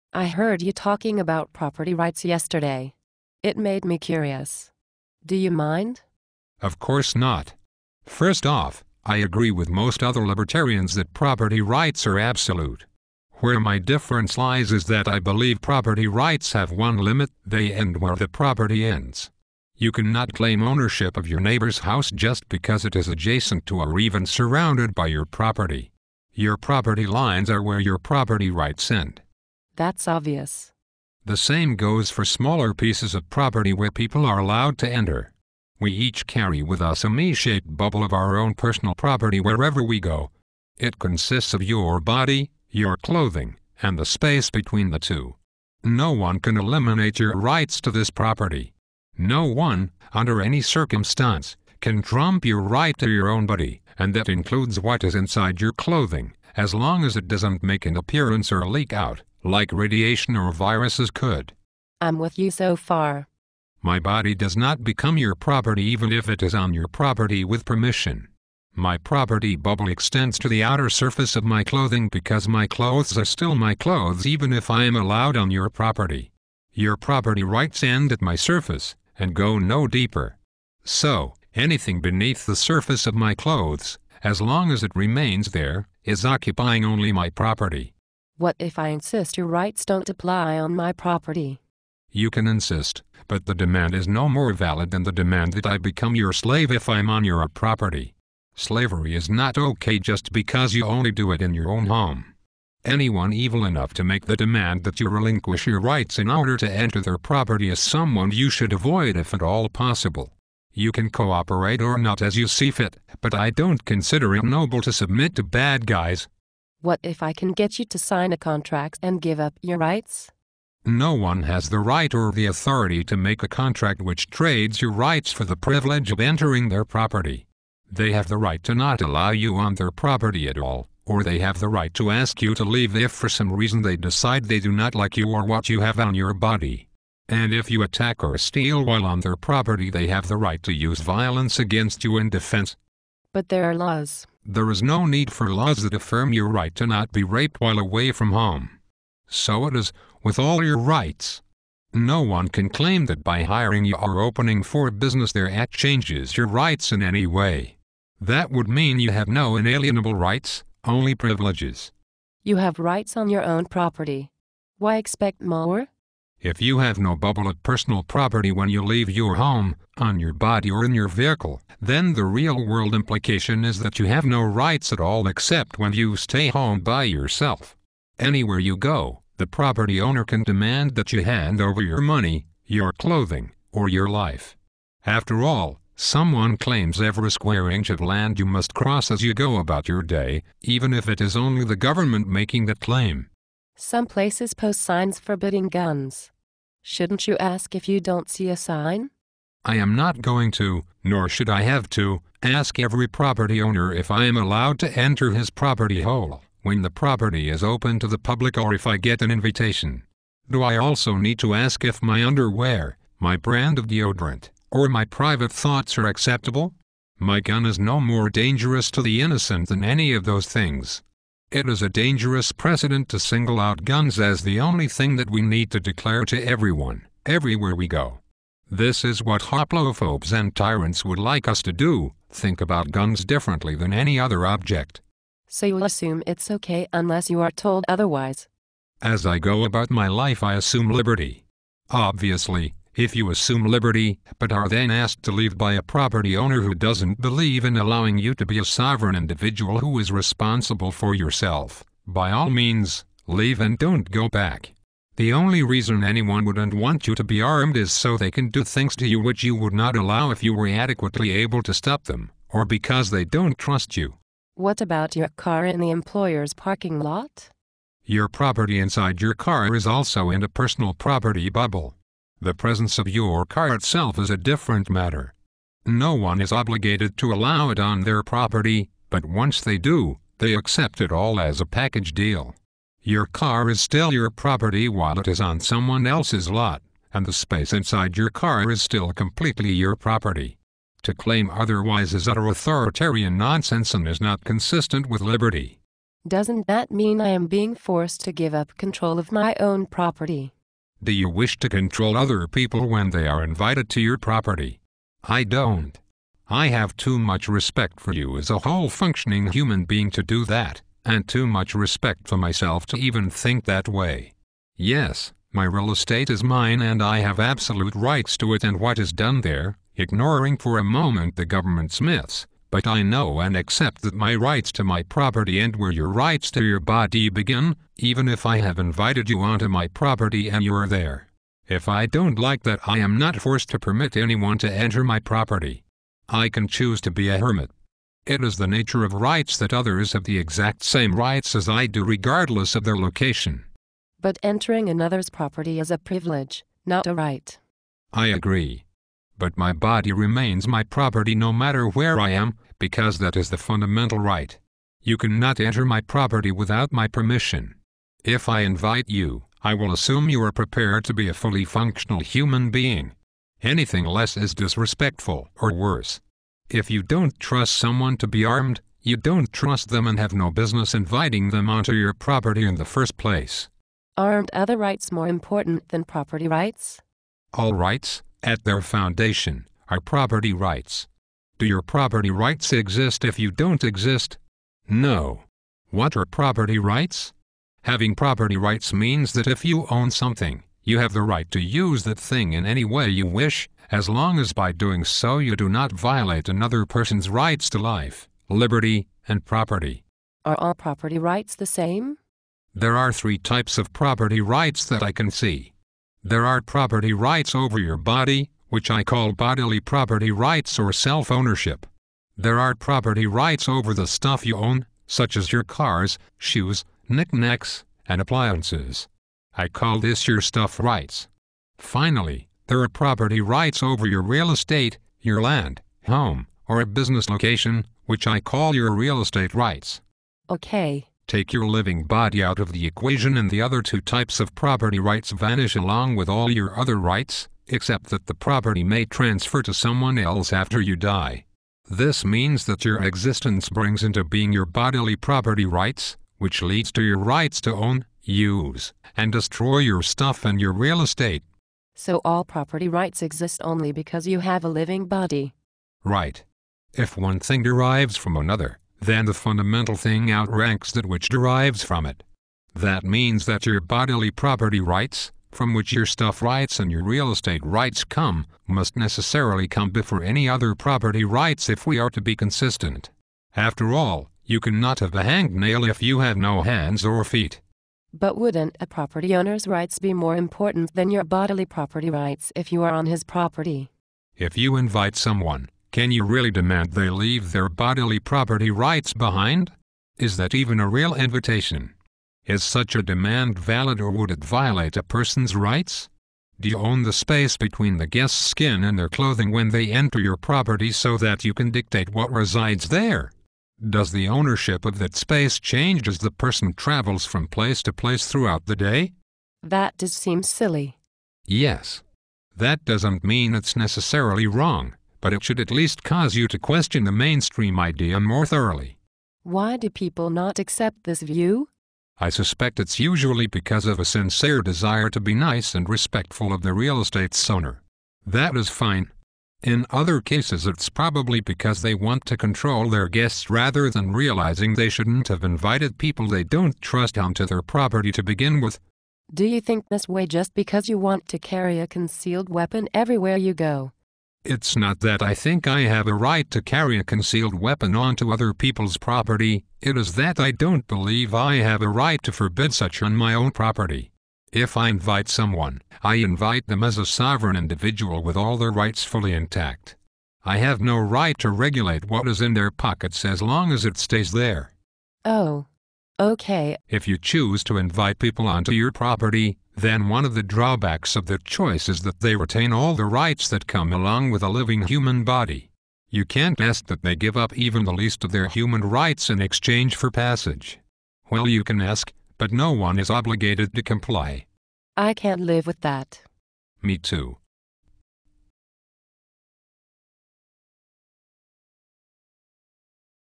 Bubble Theory of Property rights (abbreviated version)
A shorter version of the "Bubble Theory of Personal Property Rights".
I have one on the Covenant of Unanimous Consent, one on understanding Right and Wrong, one on the Zero Aggression Principle, one on "taxation", and one on the silly notion of "Love it or leave it".